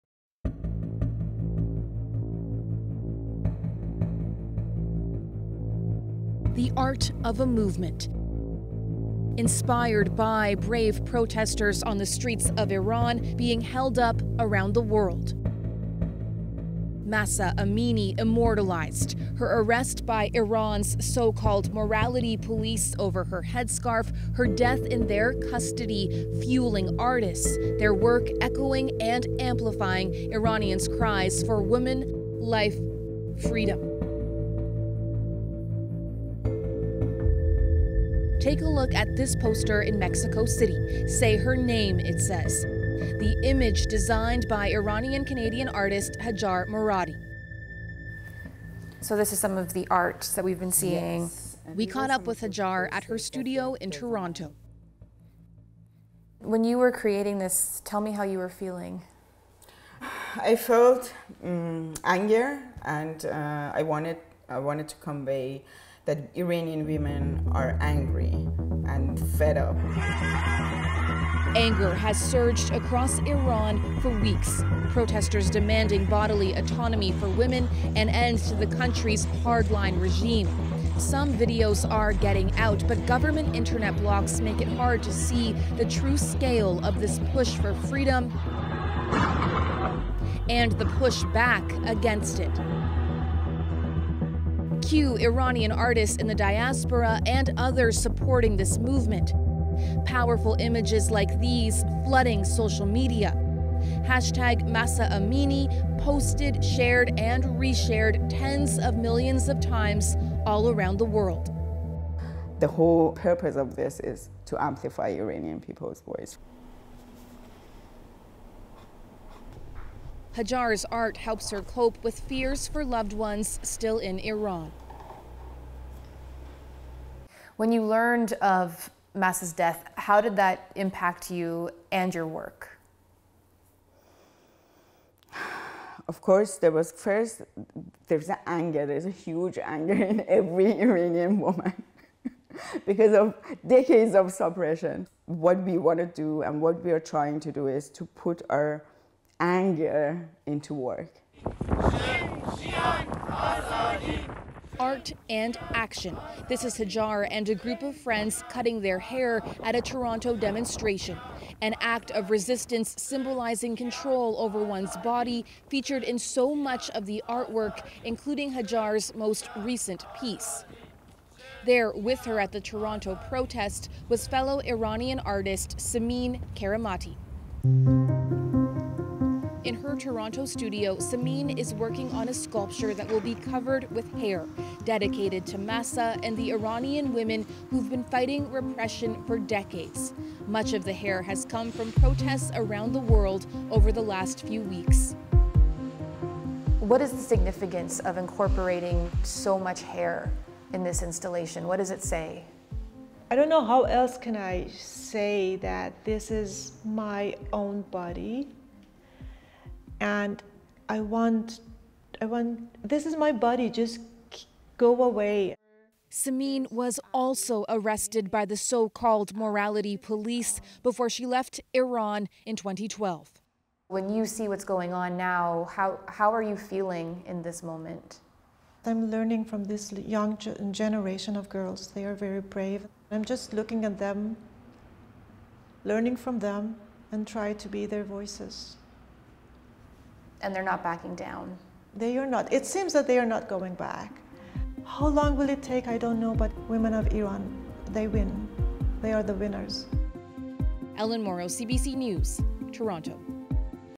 Mahsa Amini art amplifies calls for change in Iran
The arrest and death of Mahsa Amini in Iran have fuelled artists around the world, using their work to echo and amplify the demand for change.